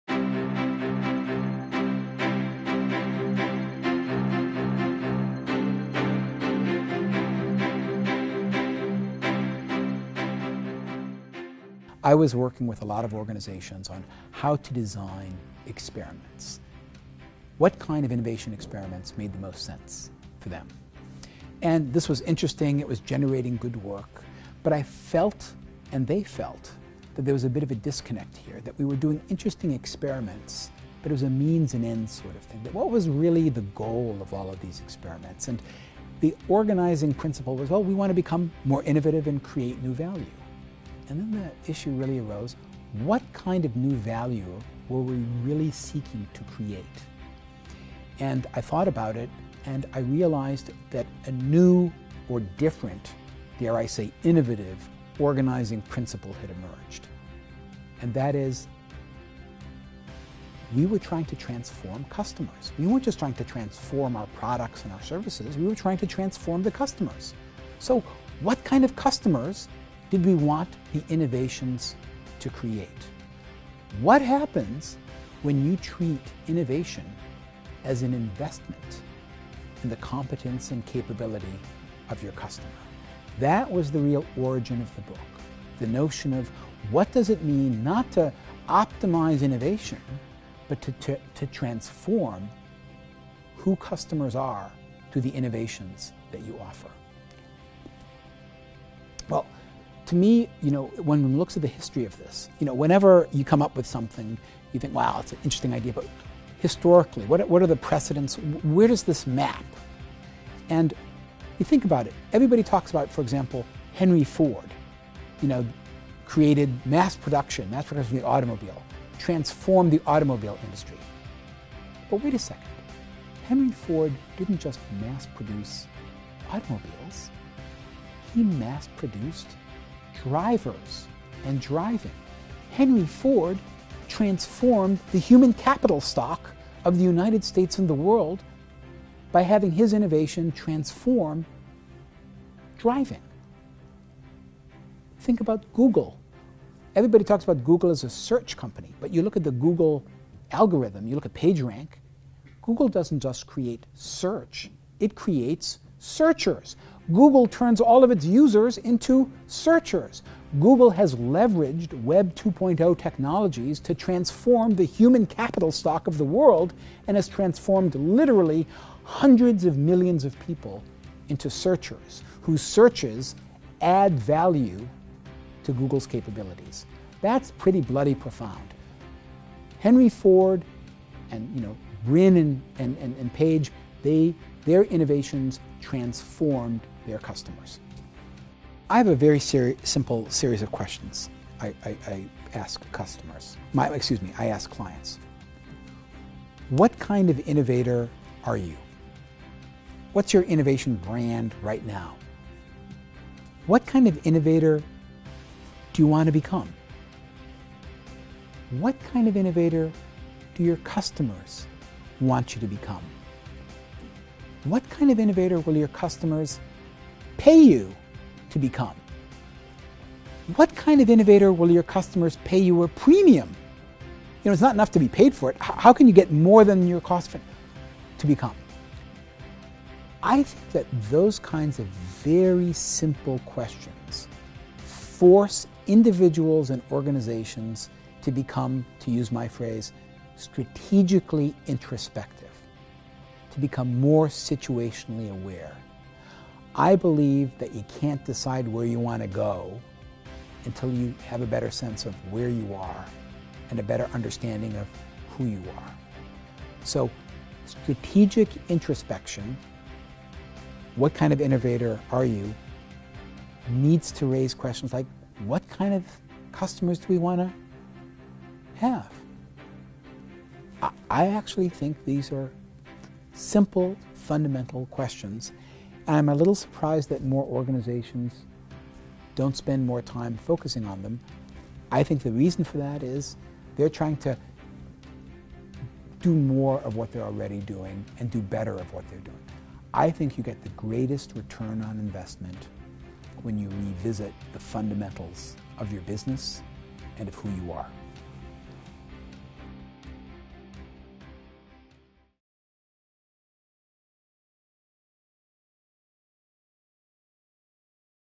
1-Michael Schrage: Transforming the Customer
MIT Center for Digital Business Research Fellow Michael Schrage discusses
Transforming the Customer